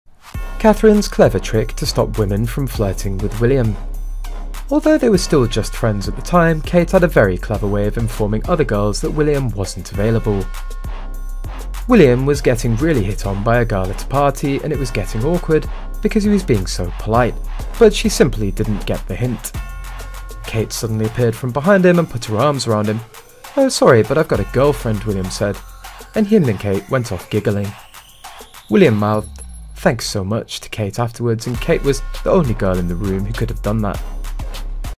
Catherine's Clever Trick To Stop Women From Flirting With William
Will was getting really hit on by a girl at a party, and it was getting awkward because he was being so polite, but she simply didn't get it. Kate suddenly appeared from behind him and put her arms around him. Oh, sorry, but I've got a girlfriend, William said, and he and Kate went off giggling."